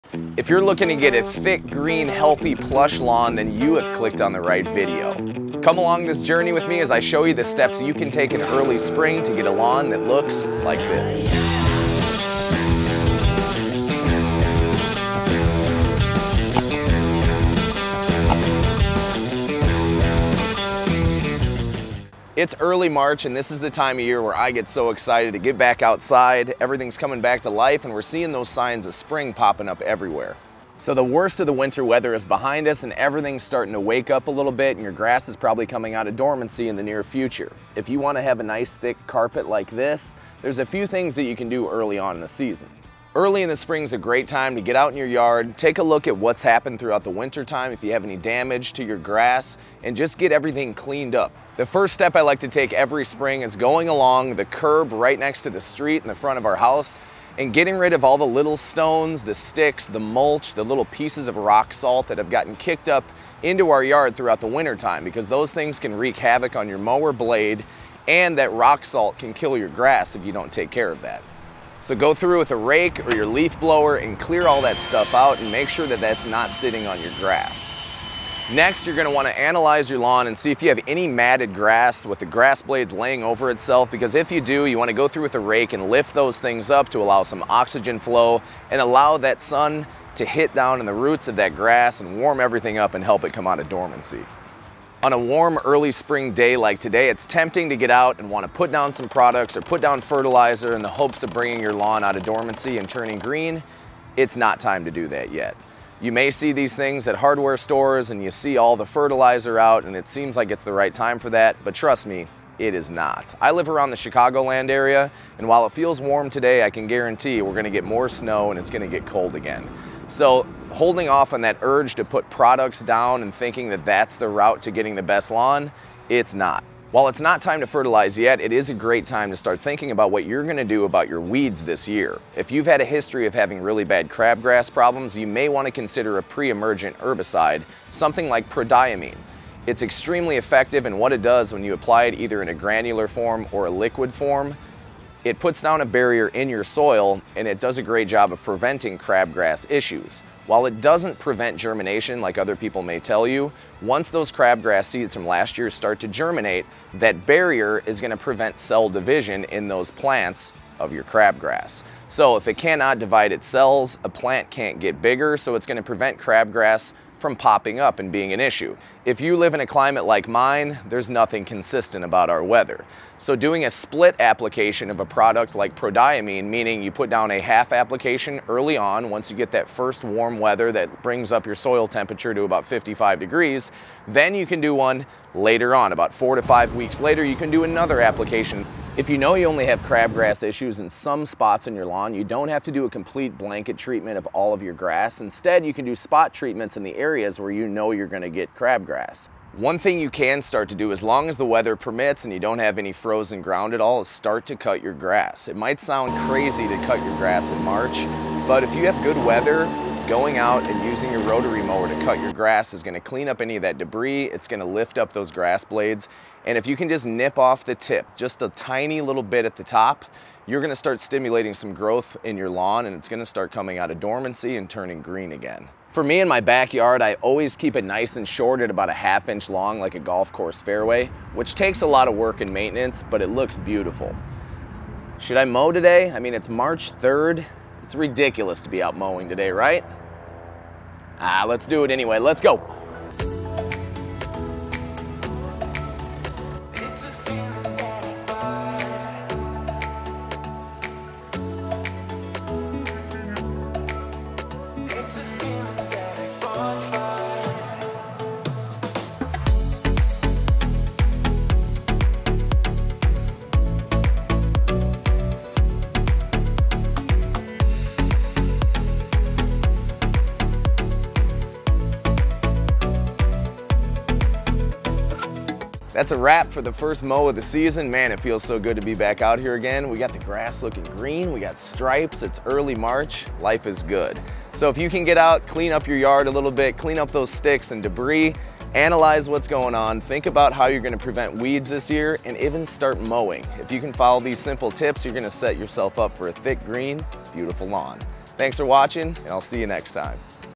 First steps to FIX YOUR LAWN
Spring is here and it's time to take your lawn to the next level! In this video, I discuss the first steps you can take to in early spring to get your lawn prepared to thrive all growing season.

👕 Want to purchase a Lakehouse Lawn Care t-shirt? Inventory is limited.